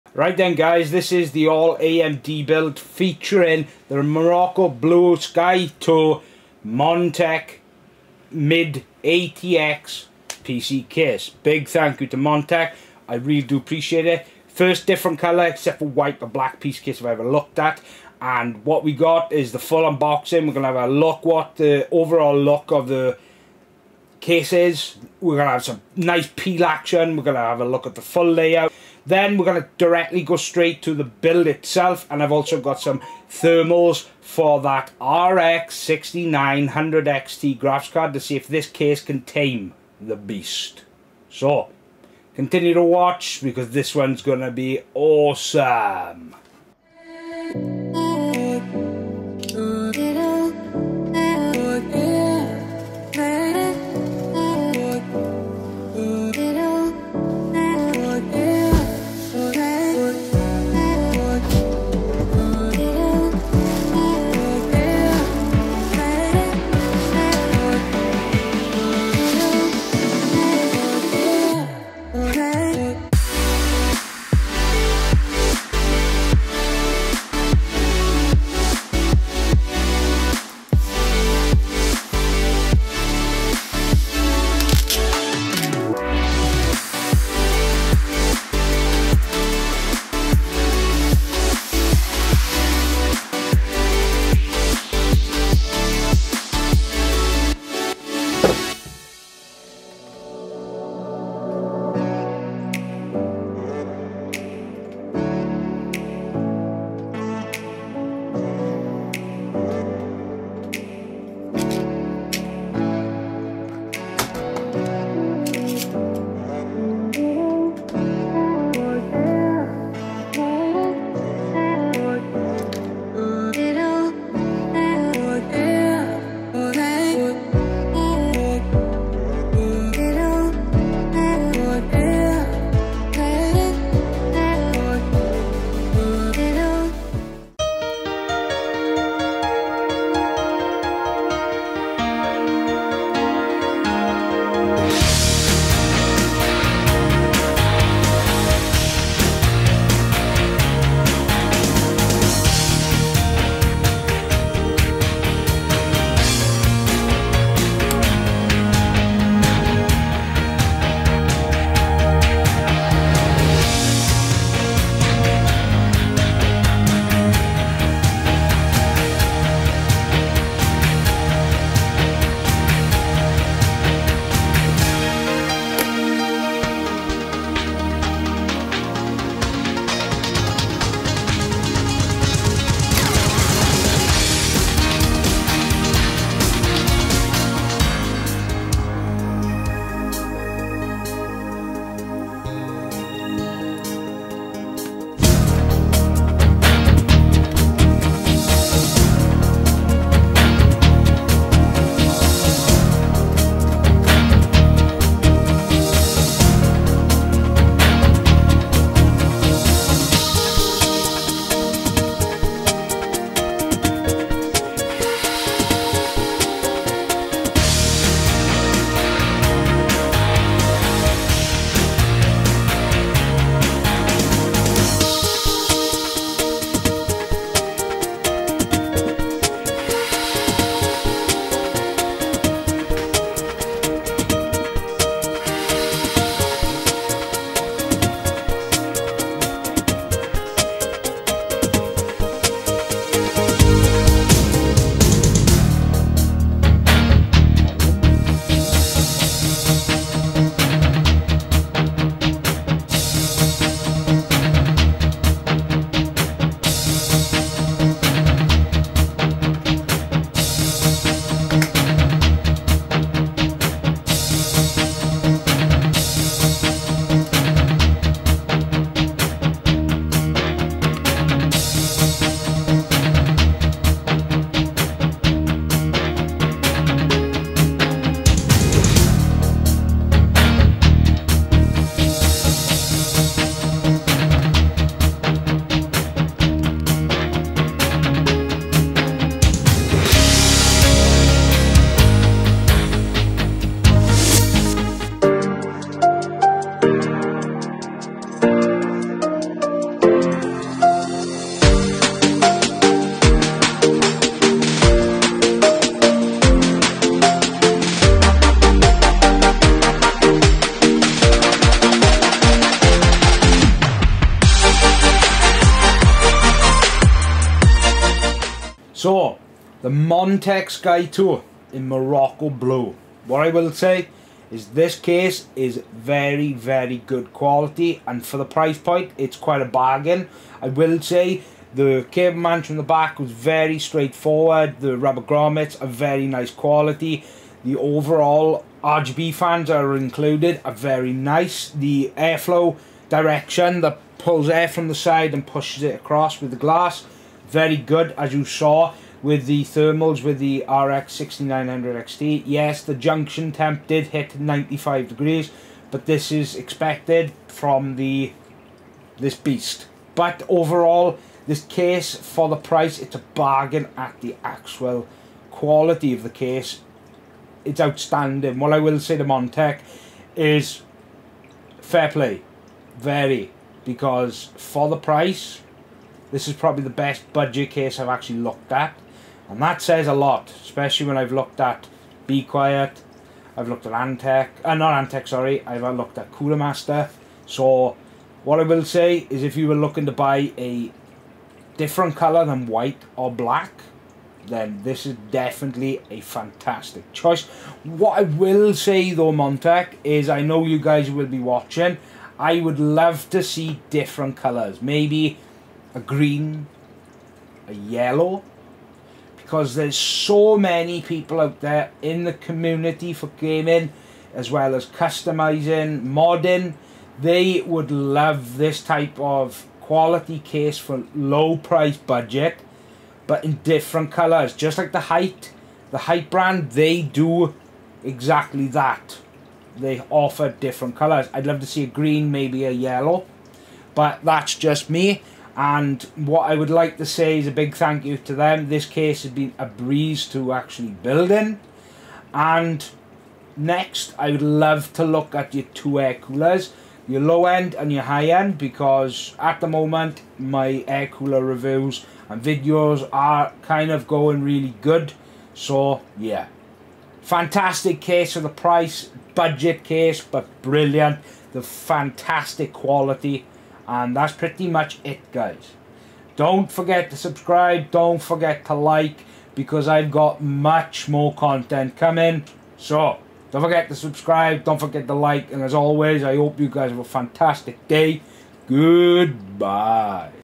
PC Build AMD RX 6900XT / Featuring Montech Sky Two Morocco Blue PC Case
Full PC Build guys the All AMD PC Build in the Montech Sky Two Morocco Blue pc case this is going to be awesome
TeamGroup T-Force DDR4 16GB
MSI Tomahawk B450
Samsung 980 500gb nvme
RX 6900XT
be quiet 650w Power Supply
00:00 Introduction
00:46 Case Full Unboxing
02:18 PC Build Cpu installation & NVME
02:52 Teamgroup DDR4 16GB installation
03:07 Thermalright Burst Assassin installation
03:49 be quiet 650w PSU installation
04:04 Motherboard installation
04:21 RX 6900XT installation
04:33 Finished Build
04:56 GPU Thermals Can this Case tame the 6900XT
05:19 My Overall Thoughts
06:15 Best budget Case ?
06:42 i want yellow or green please
07:24 i want more Cpu Coolers Montech